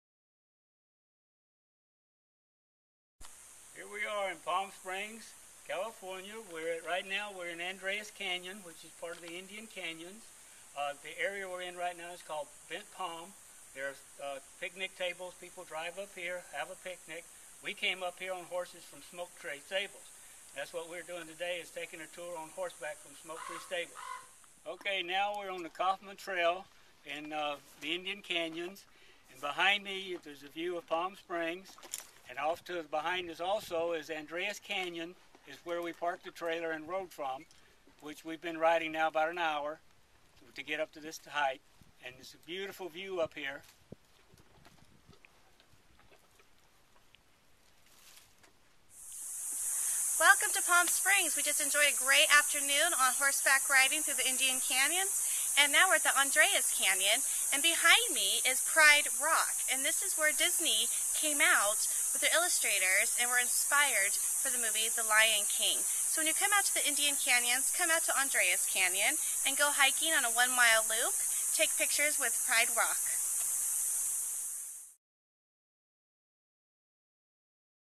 Palm Springs California Horseback Riding
Travel Writer Nancy D. Brown, goes horseback riding in Palm Springs, California with Smoke Tree Stables.  The stables offer one and two hour trail rides, or custom rides with lunch through Indian Canyons.  All riding levels welcome.